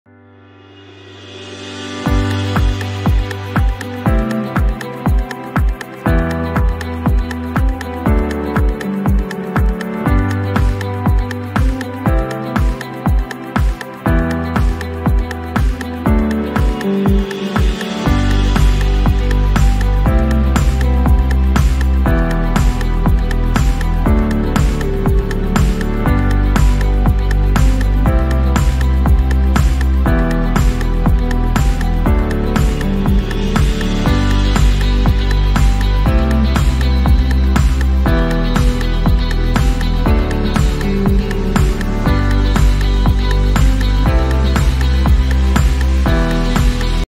Dogs playing in Doggie Daycare at Dogstown University in Deerfield Beach Florida
Dogs playing in Doggie Daycare at Dogstown University in Deerfield Beach Florida. We are a state of the art doggie daycare and dog boarding hotel located in South Florida.